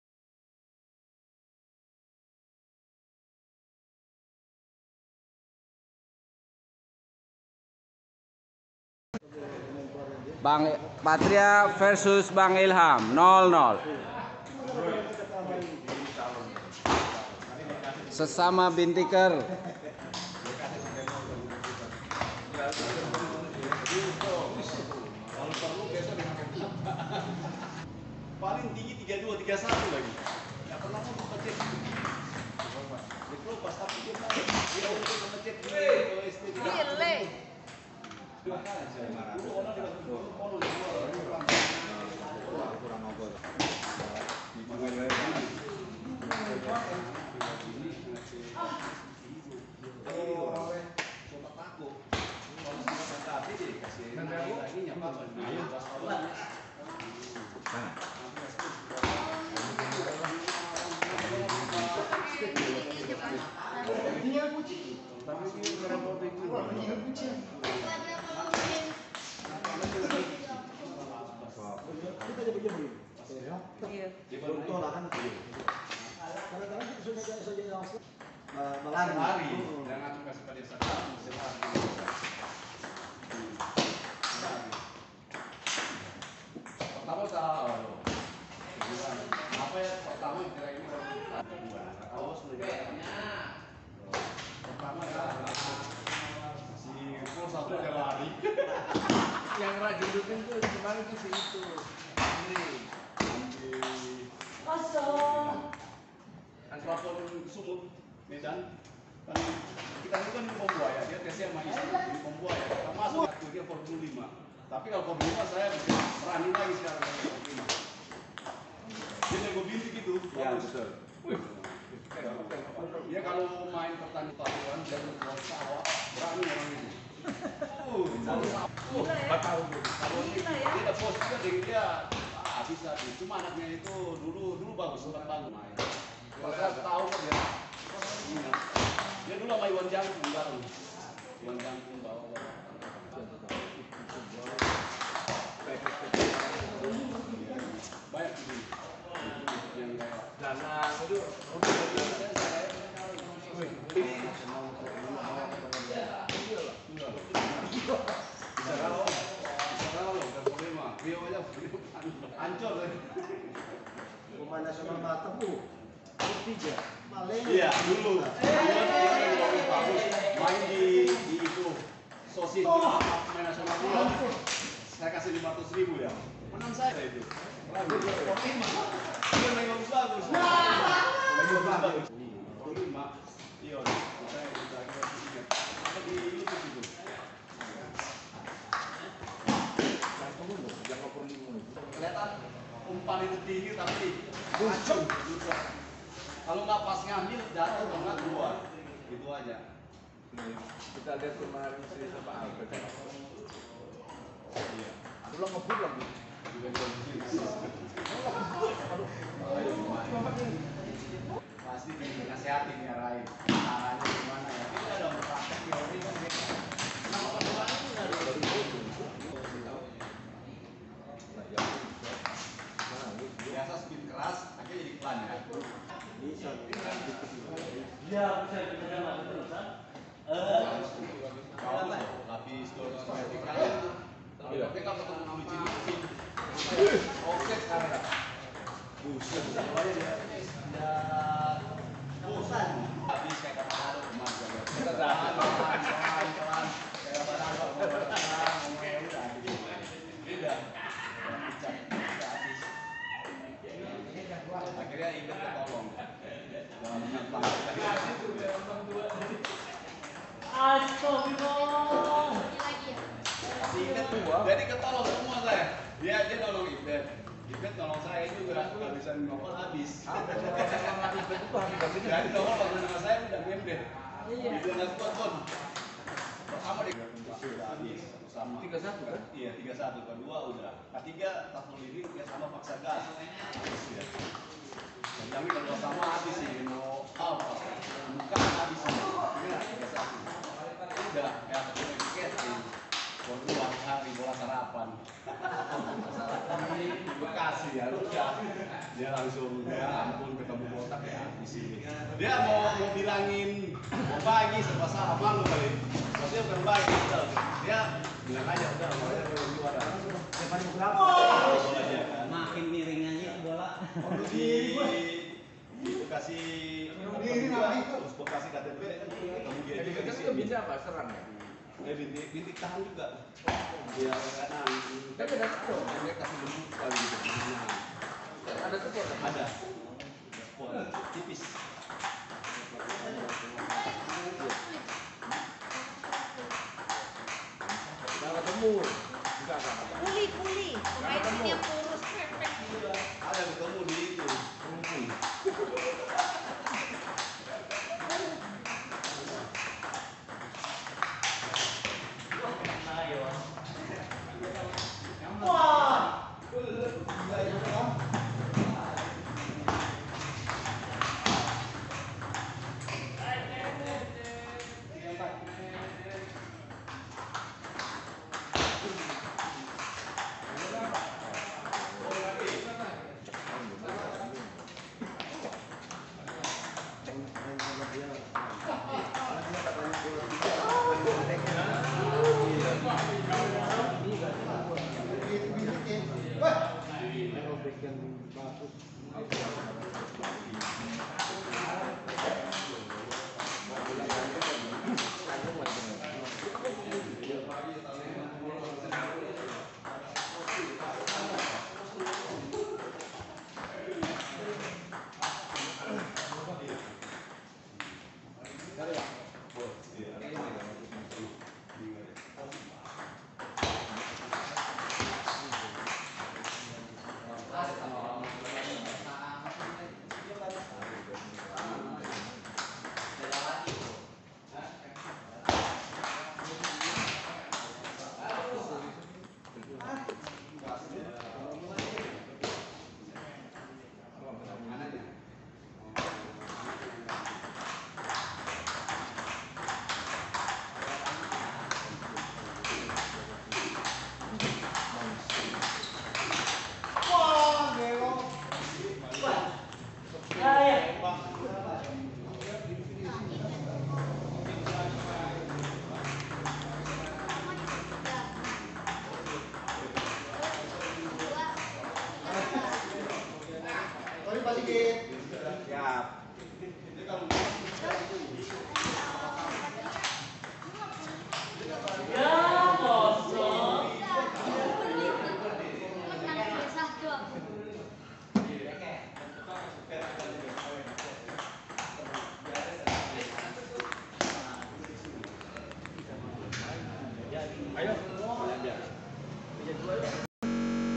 TENIS MEJA FH bintik mematikan || ILHAM vs PATRIA Prosesa || PTM Ligamas Oktober 2021
PTM Ligamas Oktober 2021 lokasi Pancoran Jaksel, tempat latihan tenis meja utk penghobby maupun Pro. Video latihan ini antara ILHAM & PATRIA  semoga menghibur, inspiratif dan edukatif utk semua pecinta tenis meja tanah air.